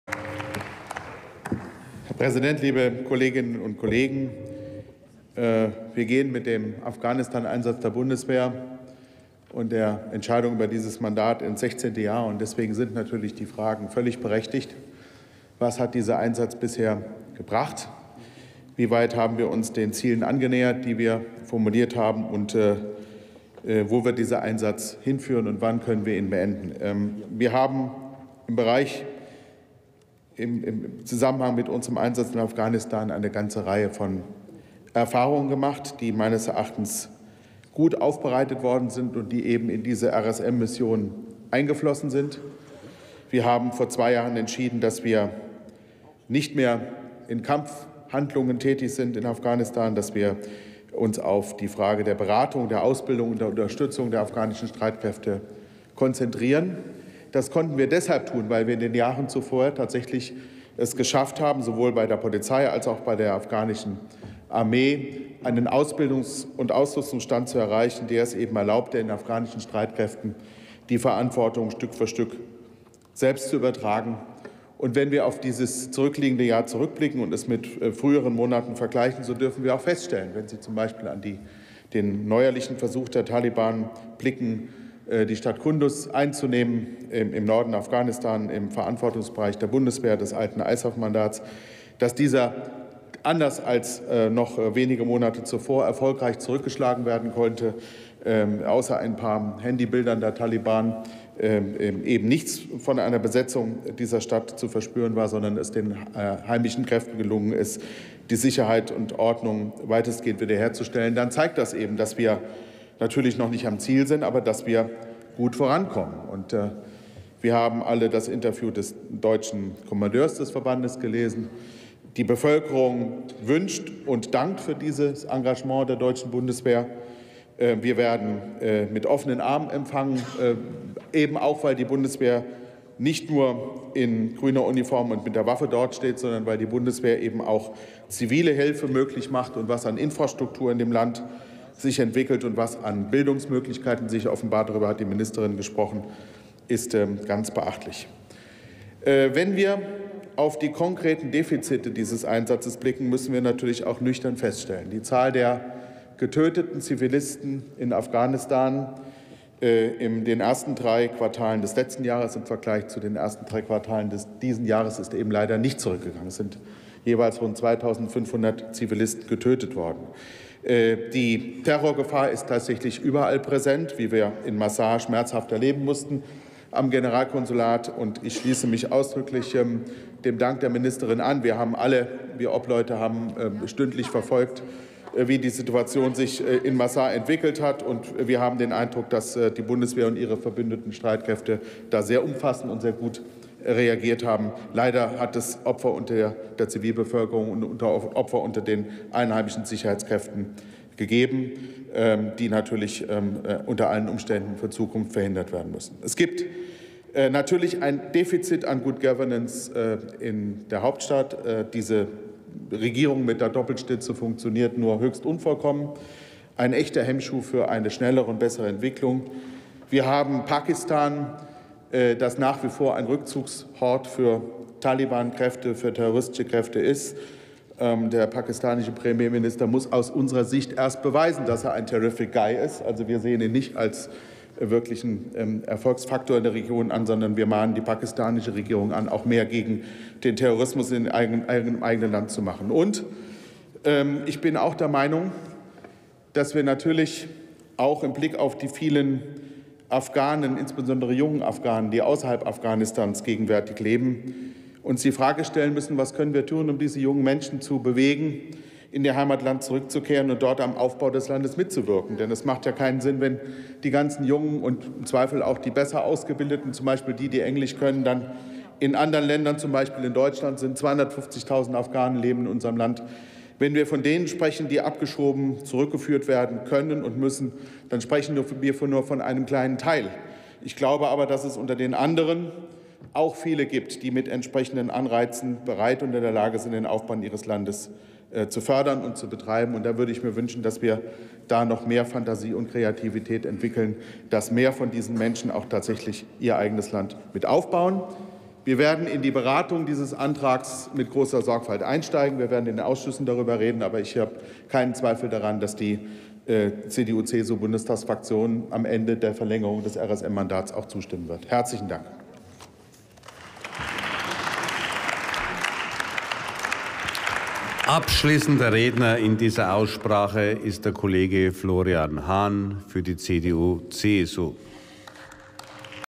Jürgen Hardt: Bundeswehreinsatz in Afghanistan [Bundestag 01.12.2016]
Fortsetzung der Beteiligung bewaffneter deutscher Streitkräfte am NATO-geführten Einsatz Resolute Support für die Ausbildung, Beratung und Unterstützung der afghanischen nationalen Verteidigungs- und Sicherheitskräfte in Afghanistan
Drucksache 18/10347

18. Deutscher Bundestag
206. Plenarsitzung
Tagesordnungspunkt: Bundeswehreinsatz in Afghanistan
Redner: Hardt, Jürgen, CDU/CSU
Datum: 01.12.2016
Plenarprotokoll:
UVID:7038766